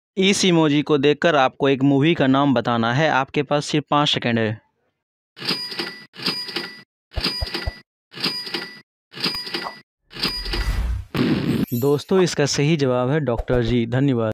मजेदार पहेली- 366 | Emojis Challenge | Can You Guess The Movie 🎥 From The Emojis?
Guess The Bollywood Movie From The Emojis Challenge?